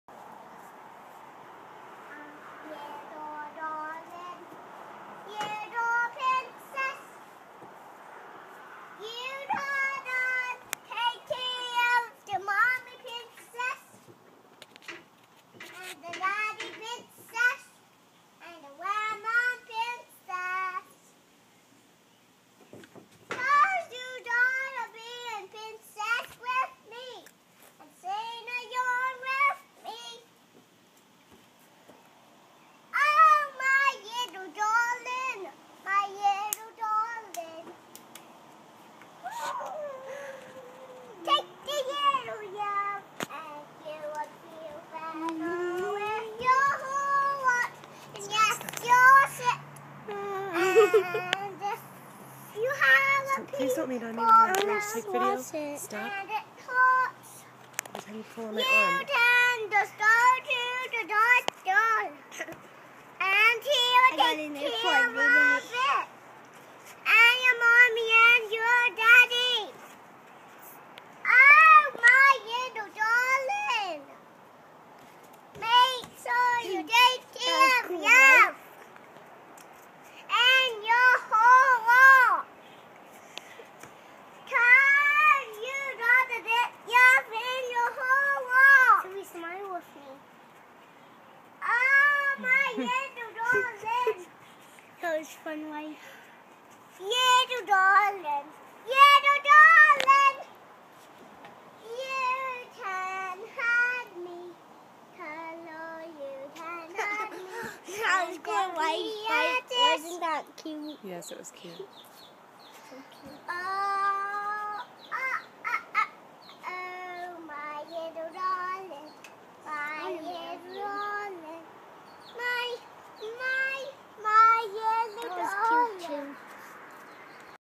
Saturday morning
Sylvie singing after Saturday morning pancakes on the back deck. Keller thought I was taking still photos.